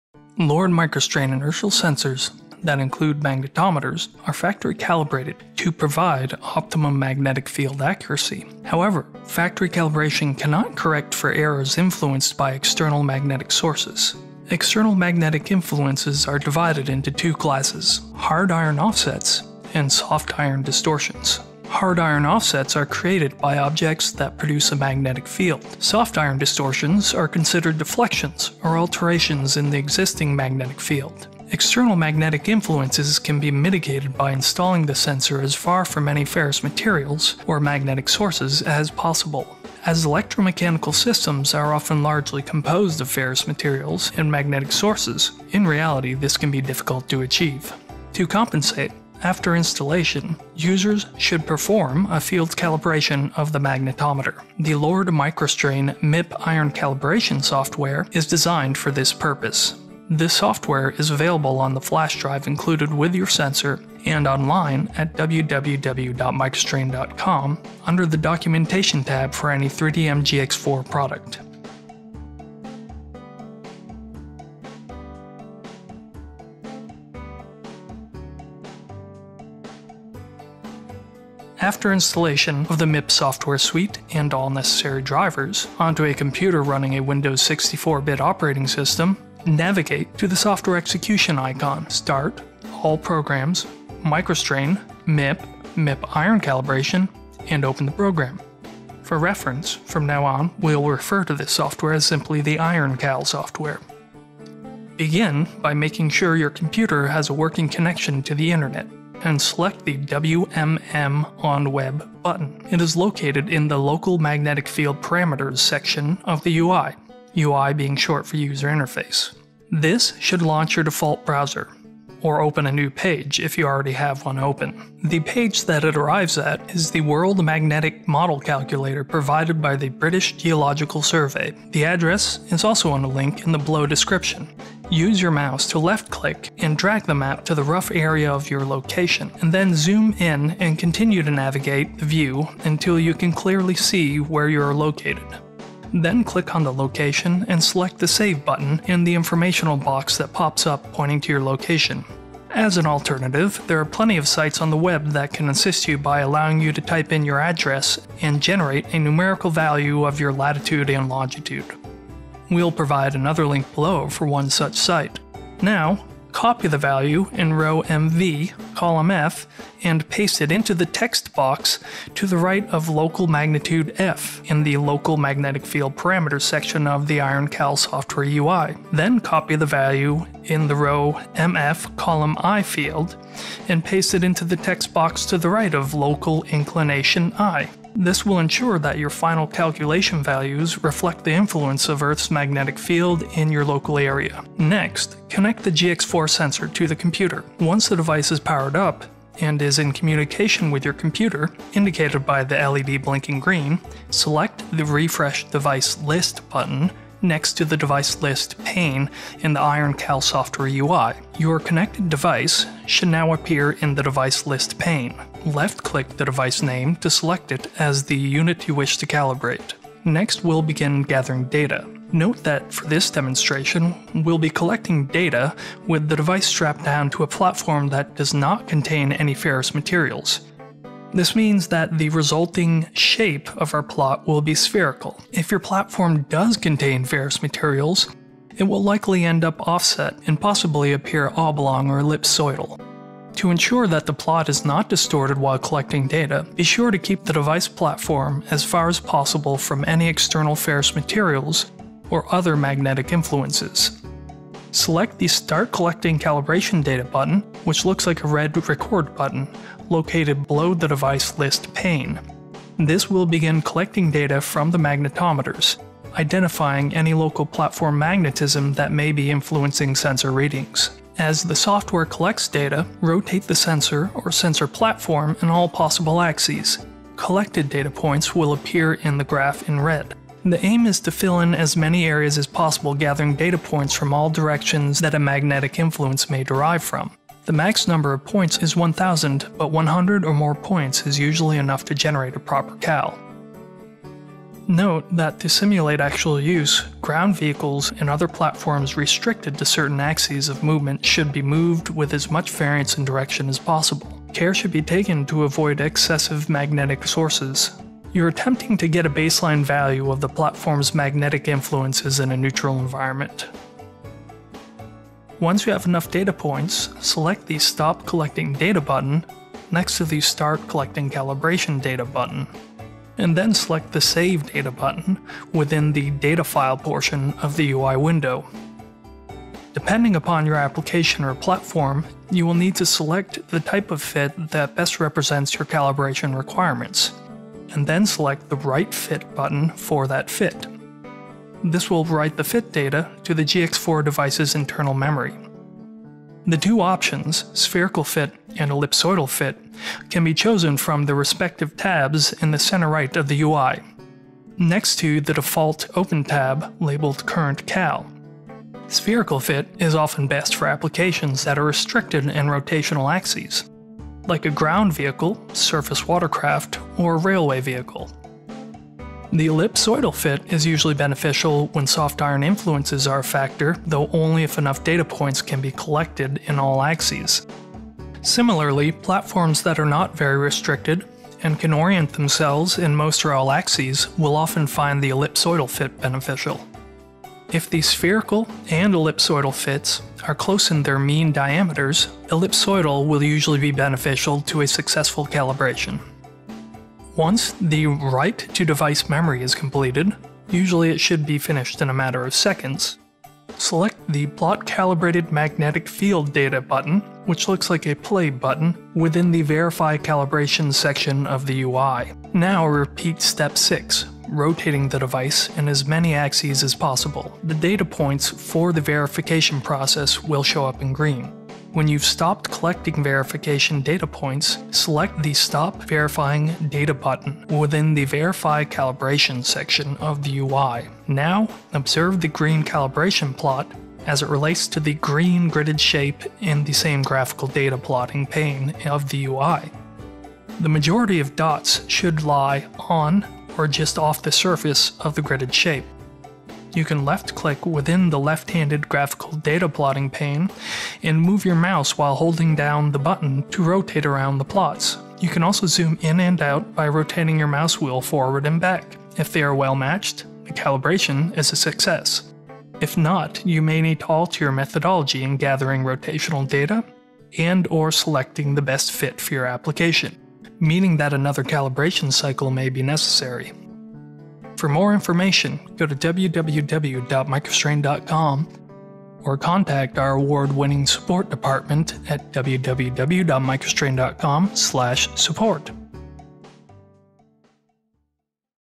MIP Iron Cal Tutorial 2014
3DM-GX4 Inertial sensor/magnetometer hard and soft Iron calibration tutorial.